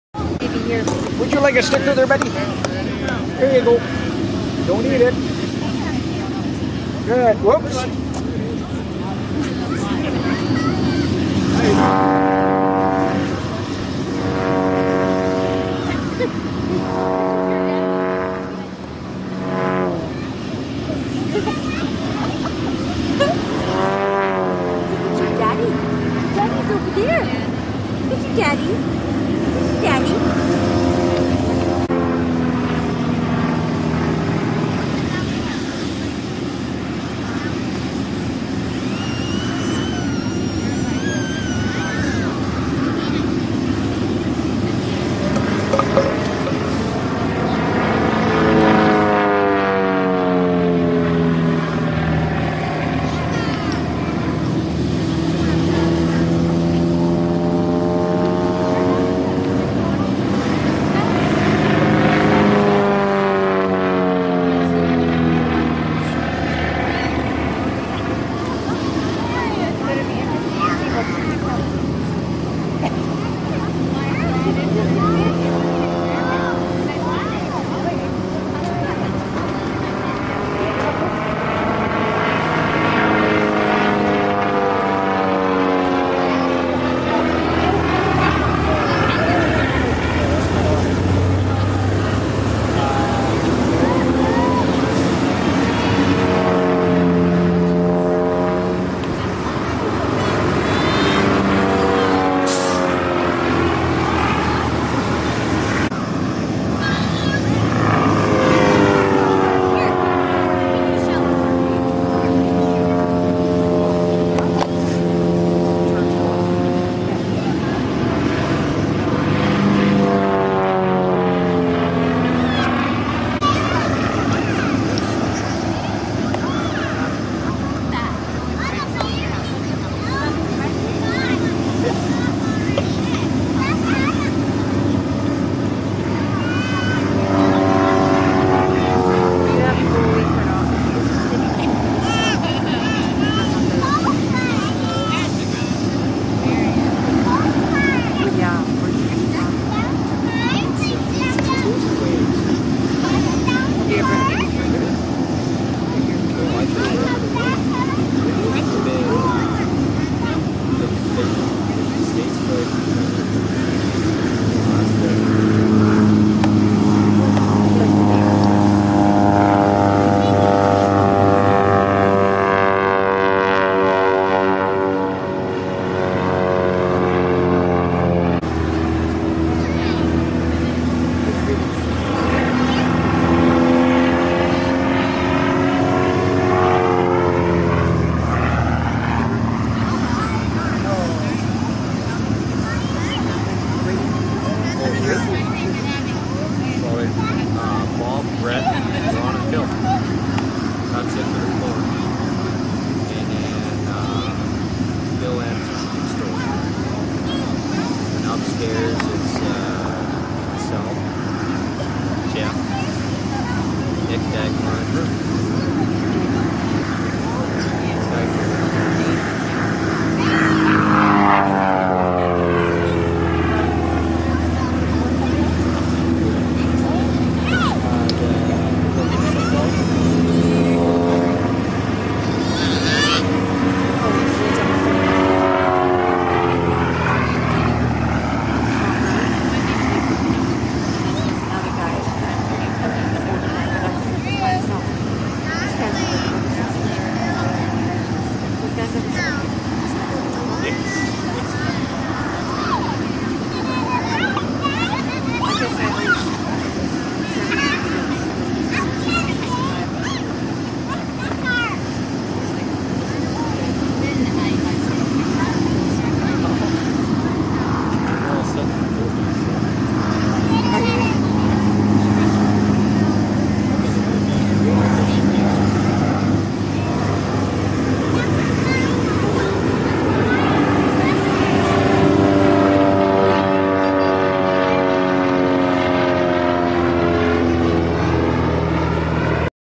Fort St. John Airshow 2007- Barry Pendrak
Barry Pendrak flies his Skybolt at FSJ 2007.